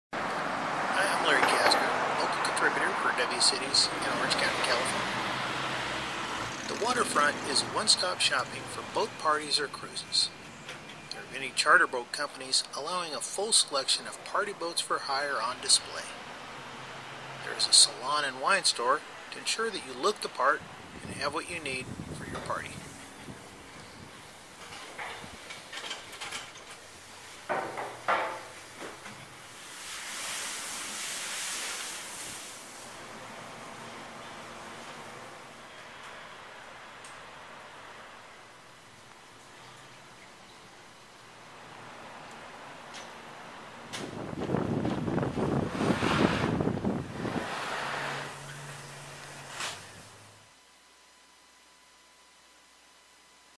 The Waterfront - Wcities
While visiting the city, one must make a stop at the Newport Beach. This destination is a water baby's paradise. Here one can indulge in all things sea; be it cruises, boat parties or seafood. The bay lights up each year during the festive season for the annual Christmas boat parade. Halloween, Thanksgiving and other great American holidays are celebrated with beach cheer here. Plan your visit at the right time of the year and carry all essentials for a fabulous beach party.